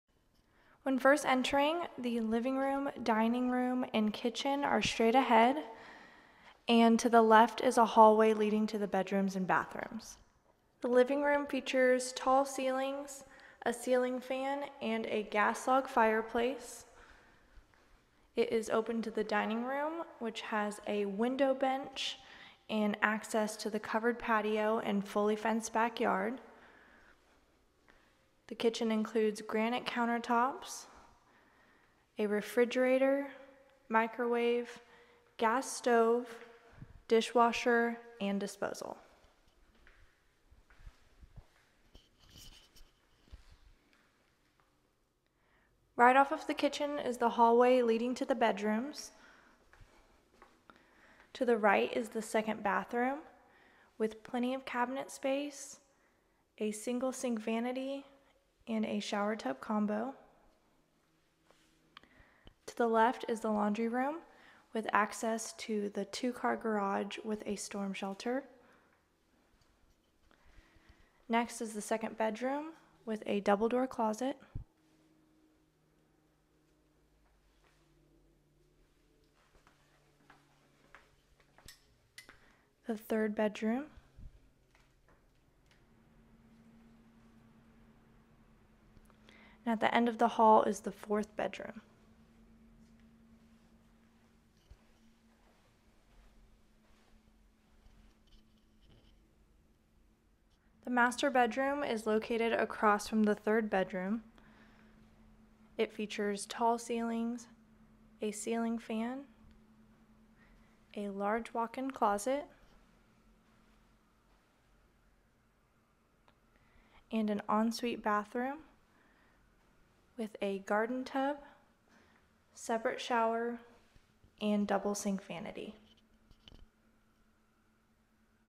3304 NW 161st St
Great 4 bed, 2 bath, 2 car home located Edmond's Ironstone addition near Portland & 164th. Wood floors in living & hall, new carpet in bedrooms (to be installed next week). Kitchen includes granite counters, eating bar & plenty of cabinets. Appliances include refrigerator, gas stove, dishwasher & microwave. The master bedroom features a large walk-in closet, ceiling fan and an en-suite bathroom with a double sink vanity plus separate garden tub and shower. Storm shelter in garage. HOA includes playground. Owner pays HOA dues. Owner will consider dog with pet deposit. Cats and smoking not permitted. Broker/Owner